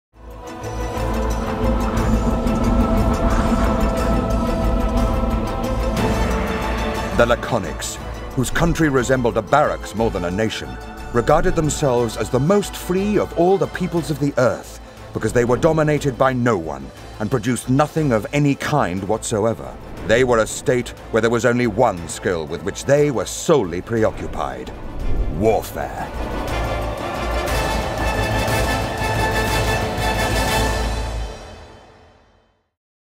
The Last Four Things - The Laconics map
Interactive video for the Penguin book "The Last Four Things".

Part of an interactive map where clicking on different sections of the map plays different videos about that area.